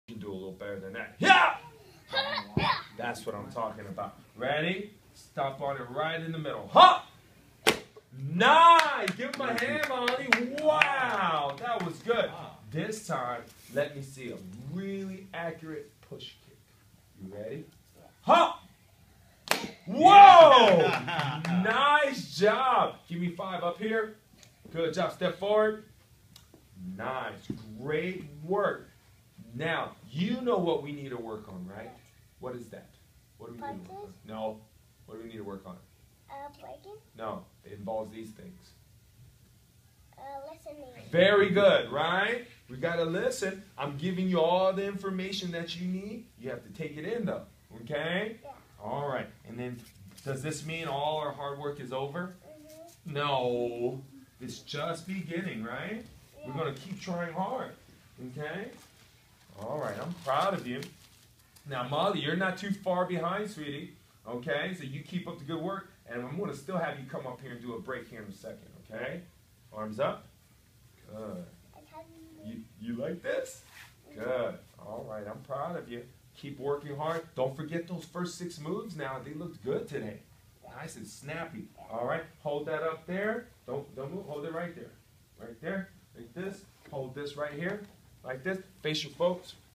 Rylan martial arts
Making up in rank to white belt with yellow stripe.